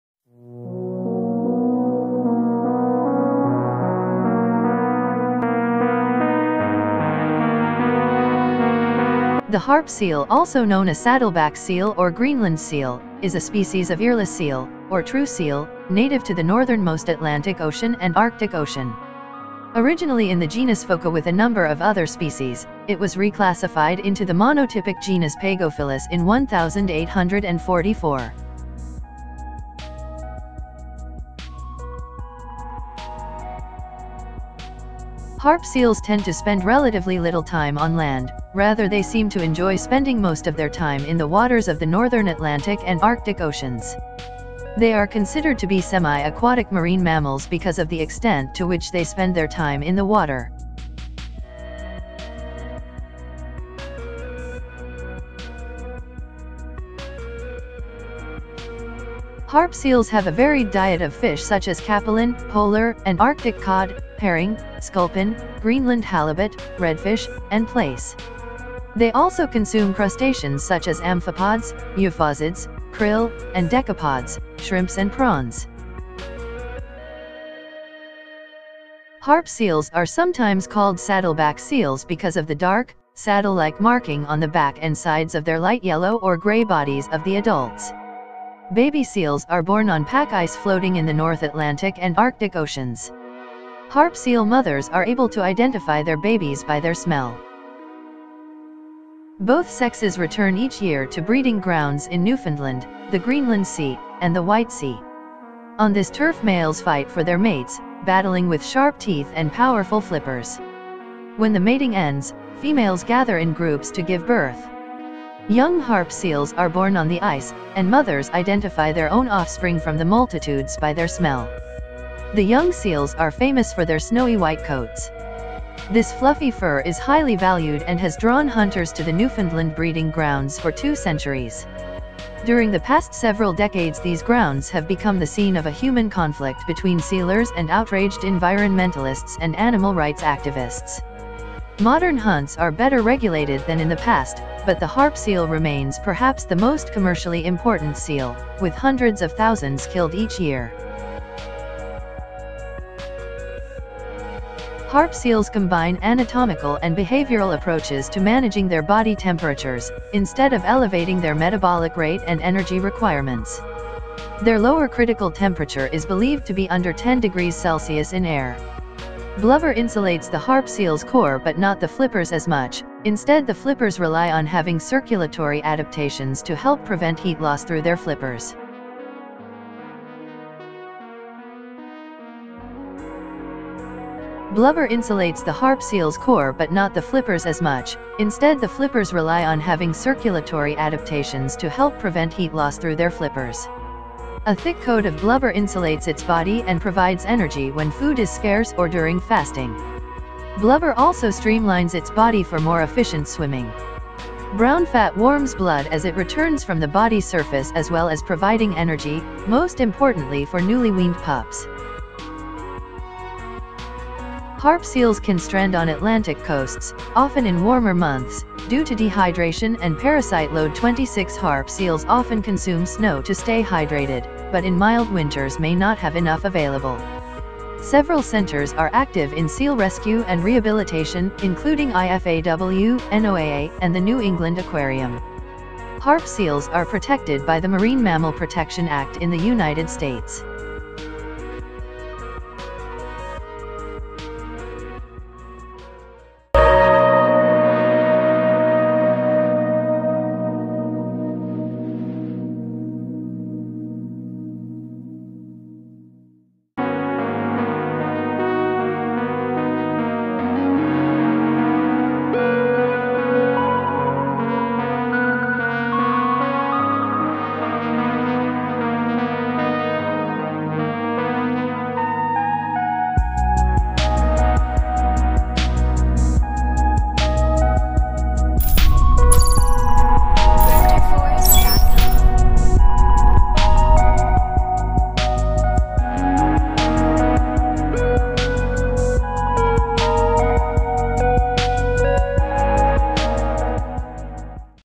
Harp Seal
Harp seals spend relatively little time on land and prefer to swim in the North Atlantic and Arctic Oceans. These sleek swimmers cruise the chilly waters and feed on fish and crustaceans. They can remain submerged for up to 15 minutes. Harp seals are sometimes called saddleback seals because of the dark, saddlelike marking on the back and sides of their light yellow or gray bodies.